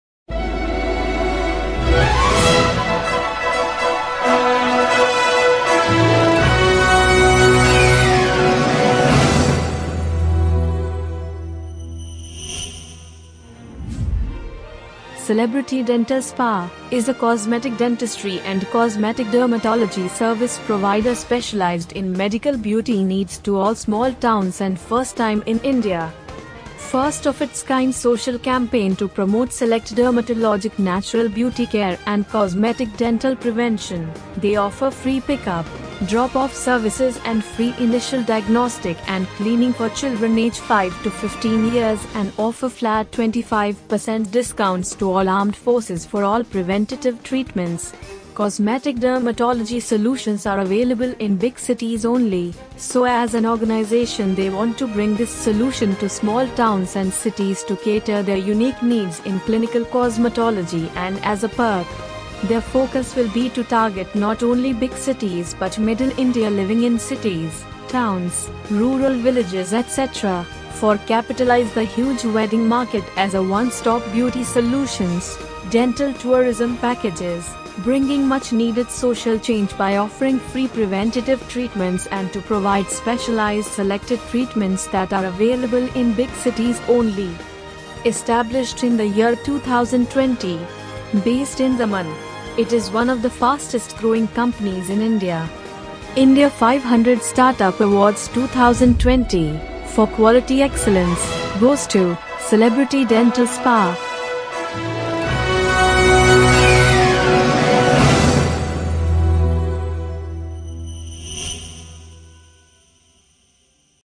Celebrity Dental Spa - Winner of India 500 Startup Awards 2020
Celebrity Dental Spa, is a cosmetic dentistry & cosmetic dermatology service provider specialized in medical beauty needs to all small towns and first time in India.

First of its kind social campaign to promote select dermatologic natural beauty care and cosmetic dental prevention, they offer free pick up/ drop off services and free initial diagnostic and cleaning for children age 5-15 yrs. & offer flat 25% discounts to all armed forces for all preventative treatments. Cosmetic dermatology solutions are available in big cities only, so as an organization they want to bring this solution to small towns and cities to cater their unique needs in clinical cosmetology and as a perk. Their focus will be to target not only big cities but middle India living in cities, towns, rural villages etc. for capitalize the huge wedding market as a one stop beauty solutions, dental tourism packages, bringing much needed social change by offering free preventative treatments, and to provide specialize selected treatments that are available in big cities only.

Established in the year 2020. Based in Daman. It is one of the fastest growing companies in India.

India 500 Startup Awards 2020, For Quality Excellence, Goes to, Celebrity Dental Spa.